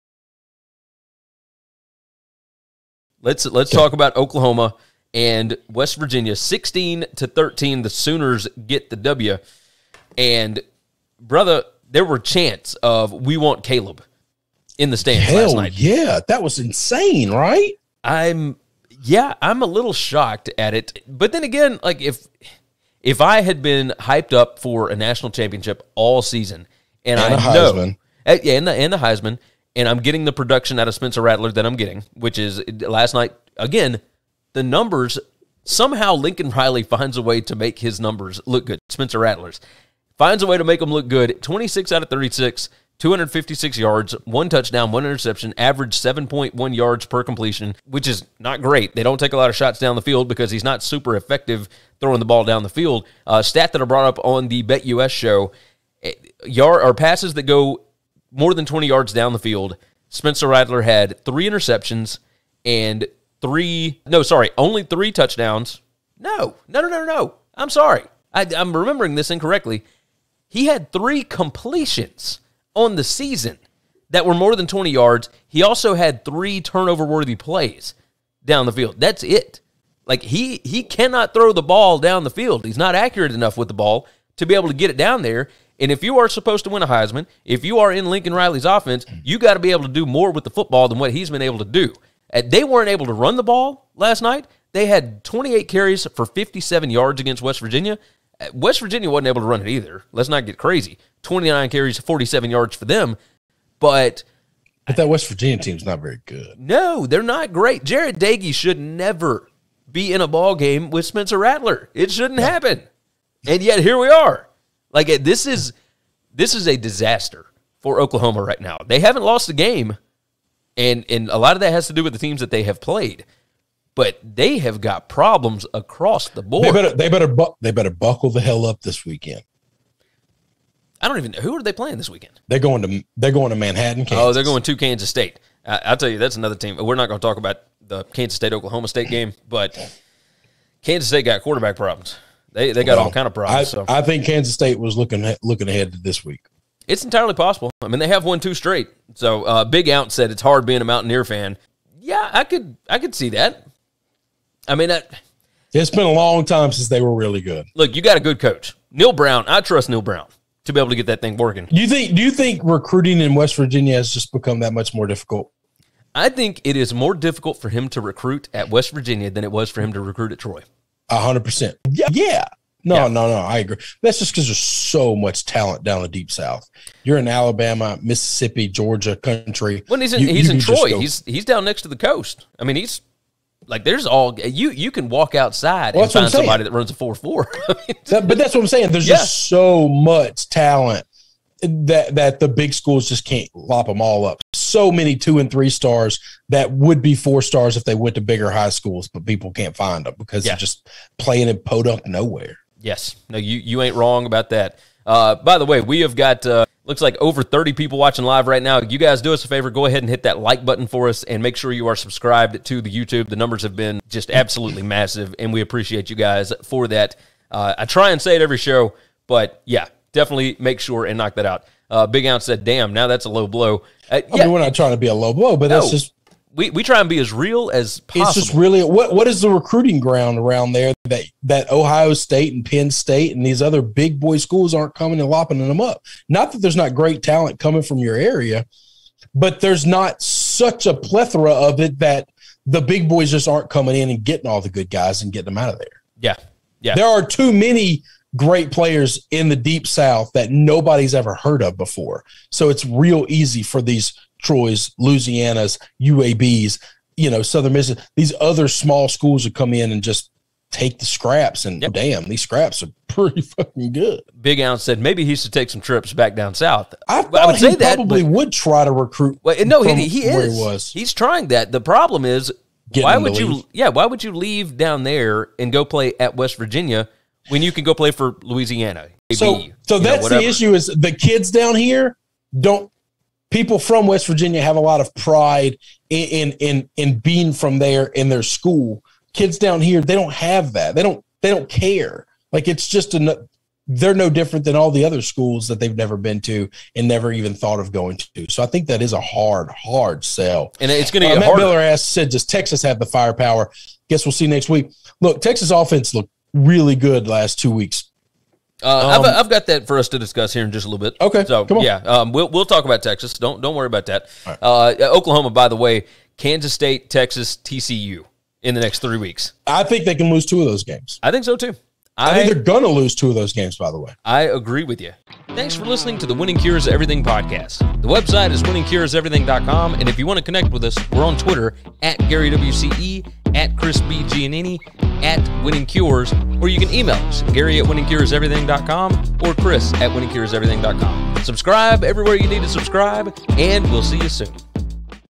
Oklahoma vs West Virginia quick reaction & recap 2021 College Football
Gary & Chris give a quick reaction, breakdown & recap to Oklahoma’s close win vs West Virginia from Week 4 of the 2021 College Football season.